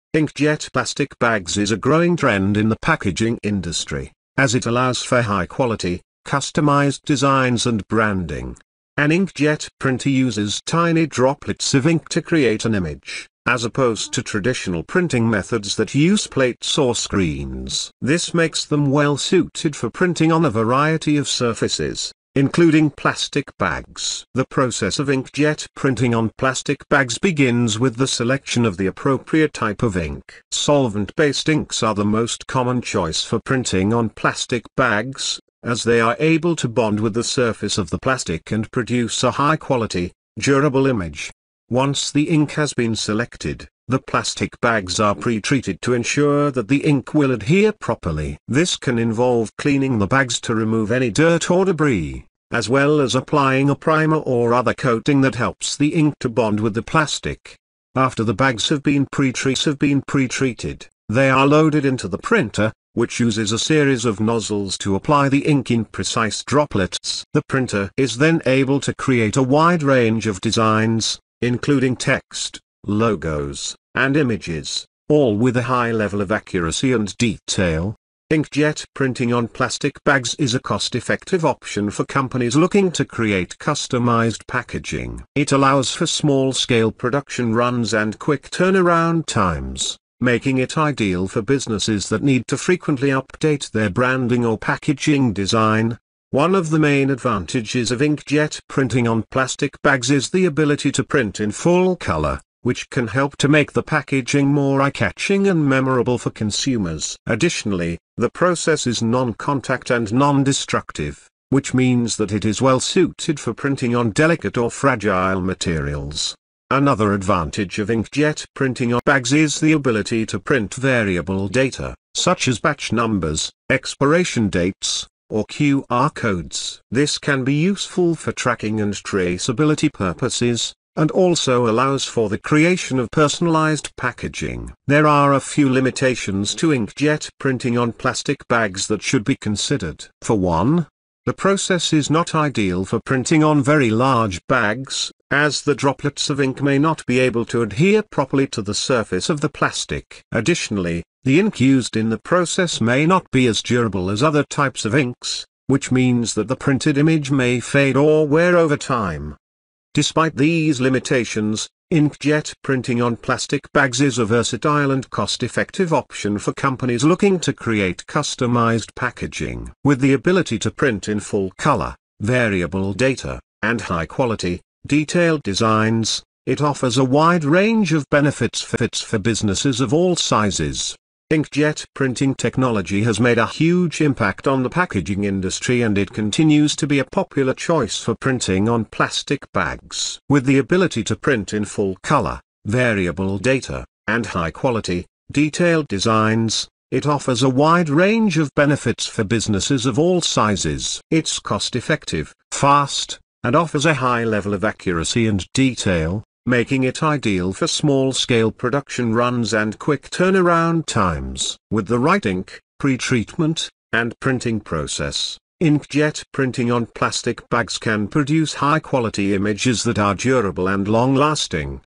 Inkjet Printer For Plastic Bags
Inkjet printers are not typically designed for printing on plastic bags. Plastic bags have a non-porous surface and can cause problems with ink adhesion and smudging. However, there are special inkjet printers available for printing on plastic and other non-porous materials. These printers use inks that are specifically formulated for non-porous materials and may produce better results. Before buying a printer, it's recommended to do research on the different options and make sure it's suitable for printing on plastic bags.
✅Top 5 Best Inkjet Printer For Plastic Bags
👉1.WELCZEK Intelligent Inkjet
👉2.BENTSAI BT-HH6105B3
👉3.TOAUTO Portable Intelligent
👉4.PEKOKO LB100 Smart Portable
👉5.OLYBSJET Portable Intelligent